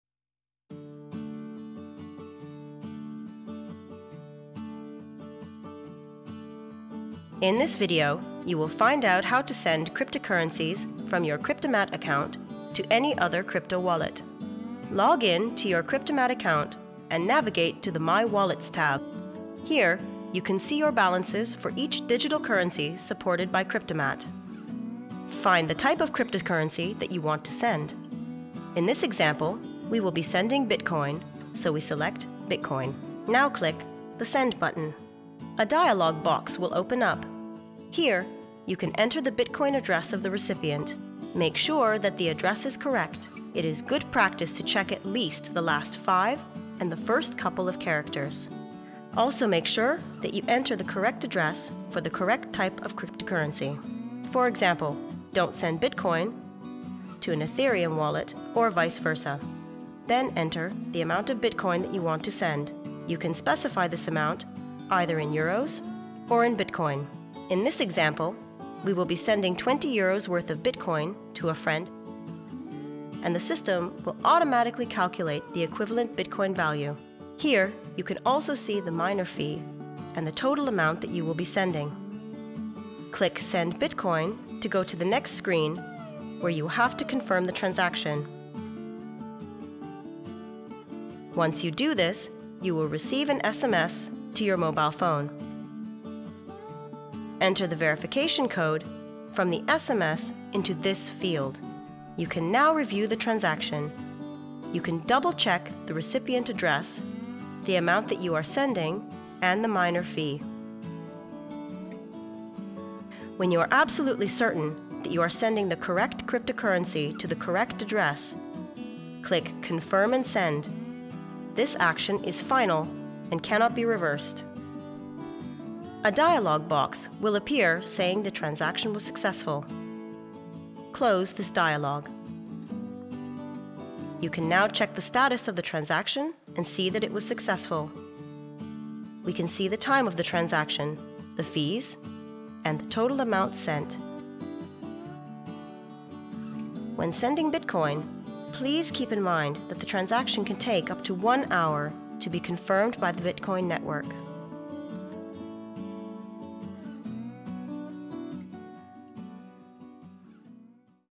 [OLD] How to Send Cryptocurrencies | Kriptomat
Find out how to send cryptocurrencies from your Kriptomat account to any other crypto wallet.

Text Instructions:
How to Send Cryptocurrencies
In this video, you will find out how to send cryptocurrencies from your Kriptomat account to any other crypto wallet.

Log in to your Kriptomat account and navigate to the My wallets tab.
Here you can see your balances for each digital currency supported by Kriptomat.

Find the type of cryptocurrency that you want to send. In this example, we will be sending bitcoin, so we select bitcoin.

Now click the Send button.

A dialog box will open up. Here you can enter the bitcoin address of the recipient. Make sure that the address is correct. It is good practice to check at least the last five and the first couple of characters.

Also make sure that you enter the correct address for the correct type of cryptocurrency. For example, don't send bitcoin to an ethereum wallet or vice versa.

Then enter the amount of bitcoin that you want to send -- you can specify this amount either in euros or in bitcoin.

In this example, we will be sending 20 euros worth of bitcoin to a friend and the system will automatically calculate the equivalent bitcoin value. Here you can also see the miner fee and the total amount that you will be sending.

Click Send Bitcoin to go to the next screen where you will have to confirm the transaction.
Once you do this, you will receive an SMS to your mobile phone.

Enter the verification code from the SMS into this field.

You can now review the transaction. You can double check the recipient address, the amount that you are sending, and the miner fee.

When you are absolutely certain that you are sending the correct cryptocurrency to the correct address, click Confirm and Send. This action is final and cannot be reversed.

A dialog box will appear saying the transaction was successful. Close this dialog.

You can now check the status of the transaction and see that it was successful. We can see the time of the transaction, the fees, and the total amount sent.

When sending bitcoin, please keep in mind that the transaction can take up to one hour to be confirmed by the bitcoin network.